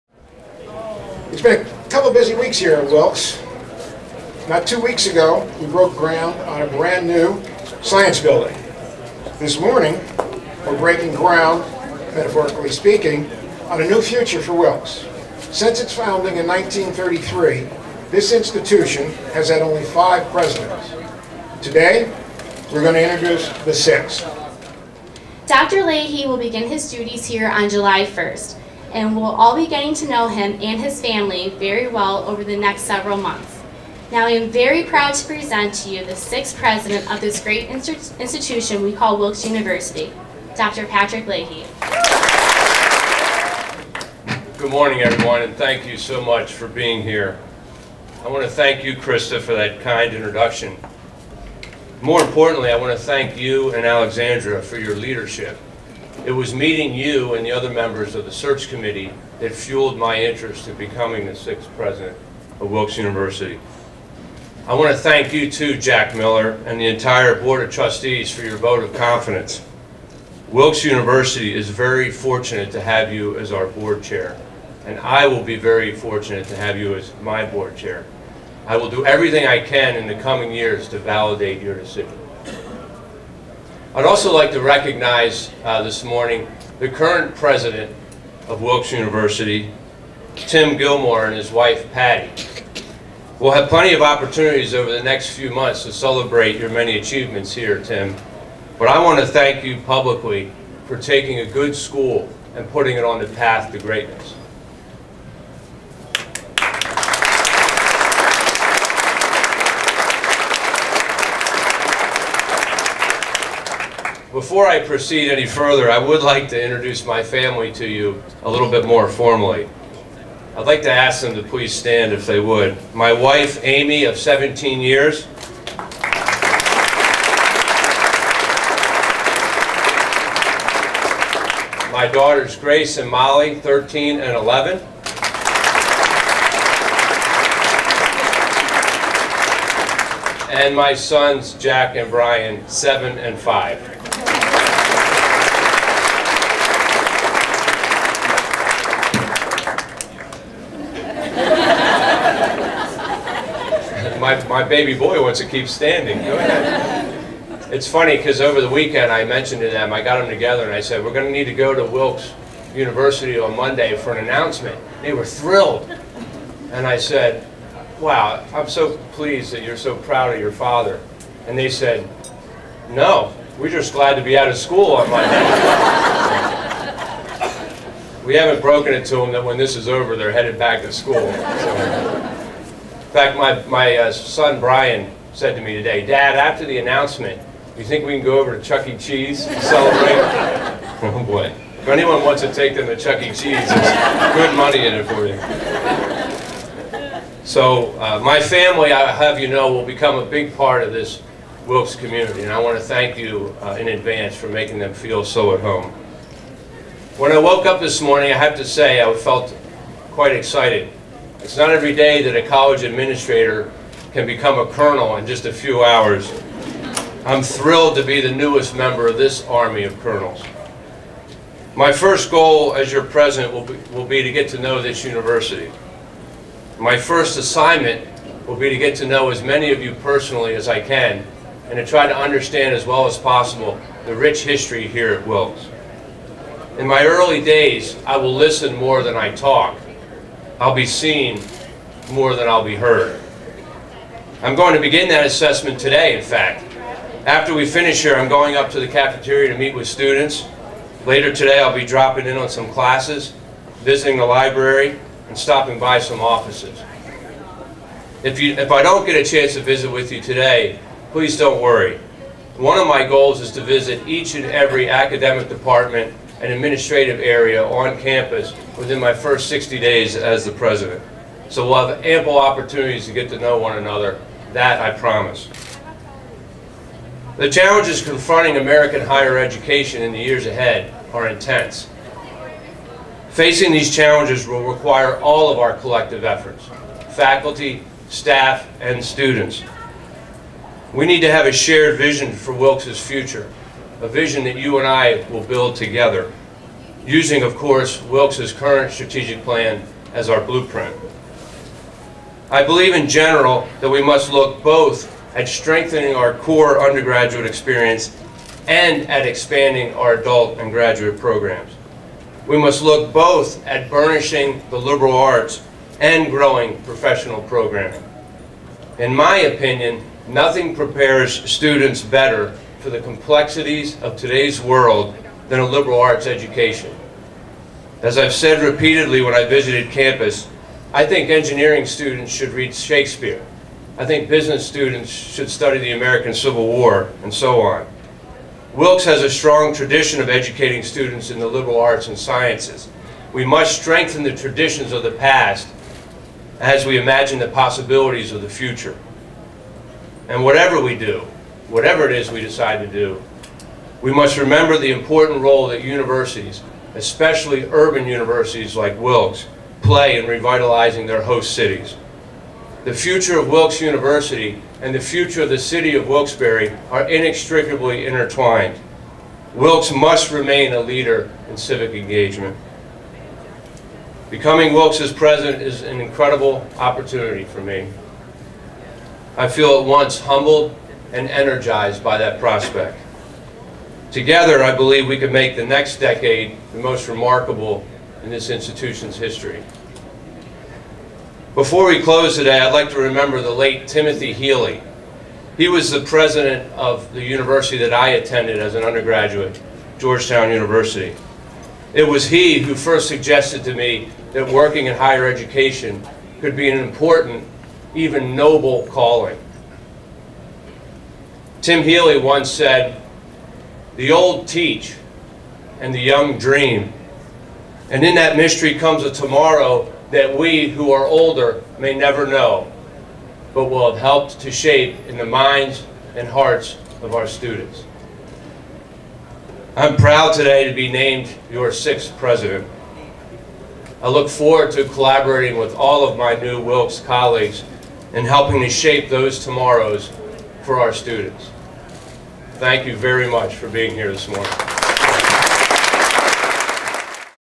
The 6th President of Wilkes University is introduced
Meet the 6th President of Wilkes University - Dr. Patrick F. Leahy